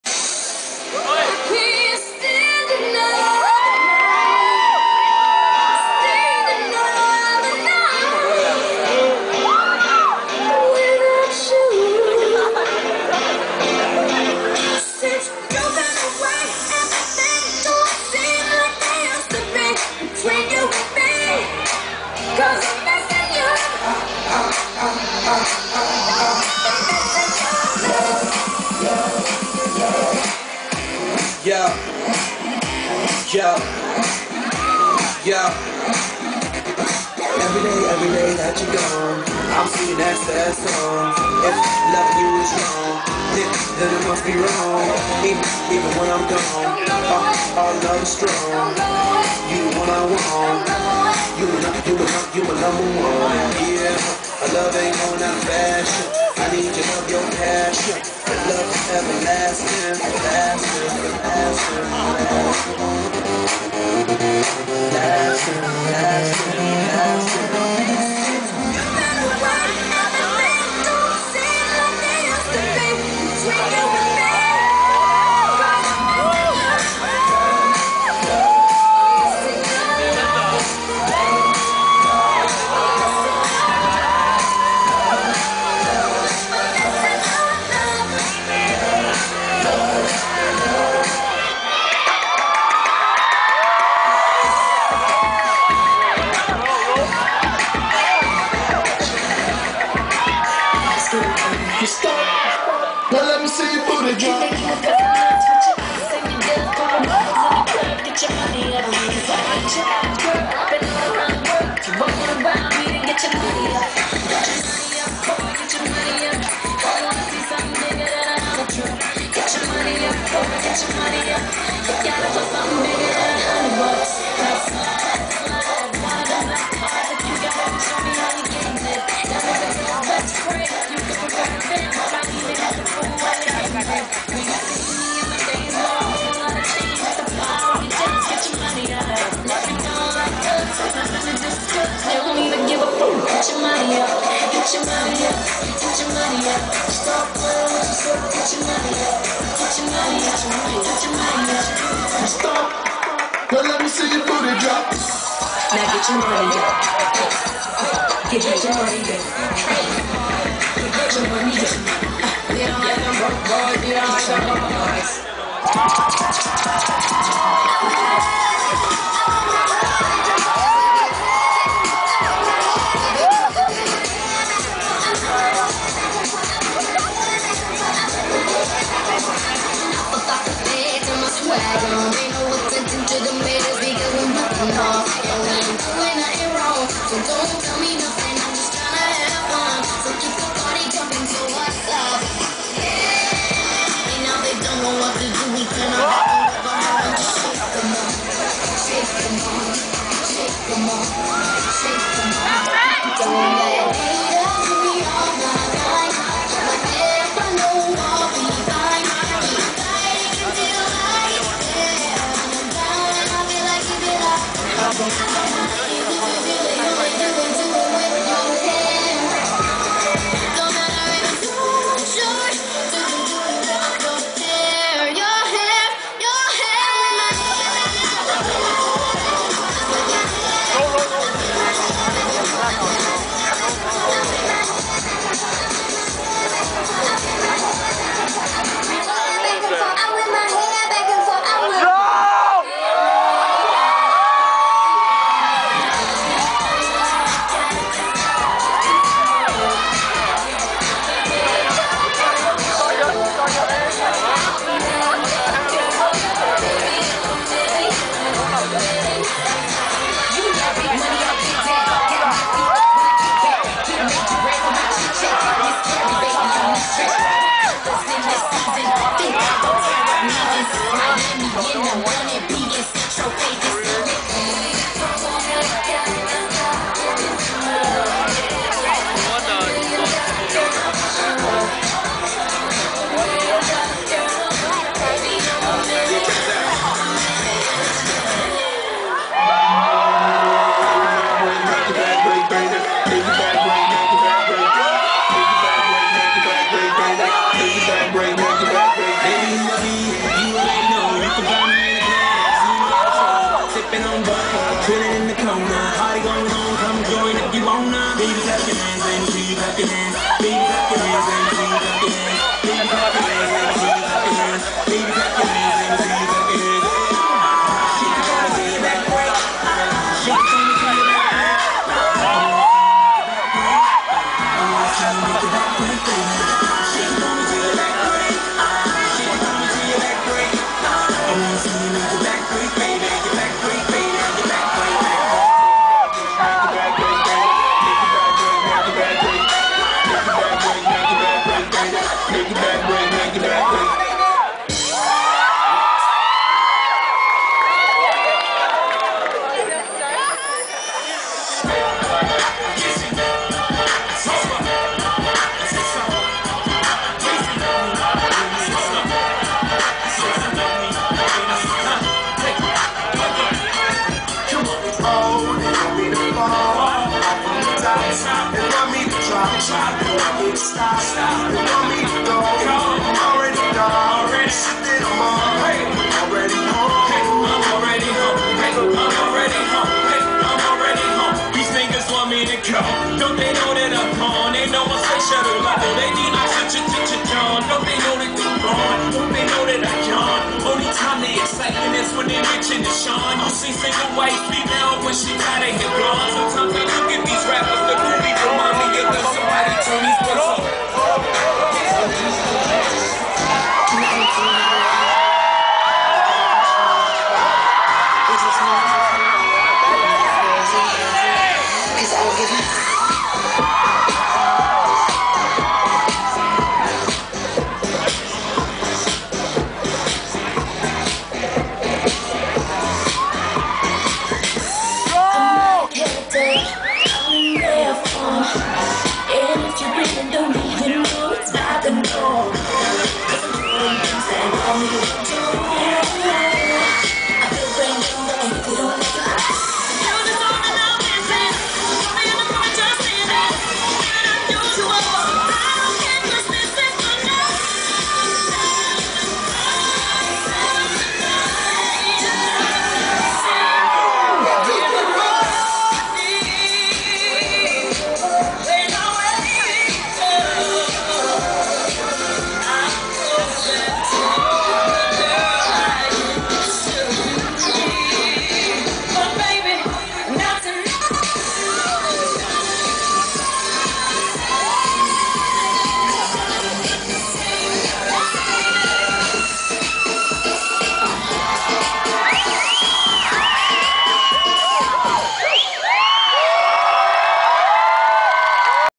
Flare Dance Ensemble at the 2011 MUOSS Night Market
A performance by the University of Melbourne Flare Dance Ensemble at the 2011 MUOSS Night Market, held at Melbourne Uni - April 7th 2011.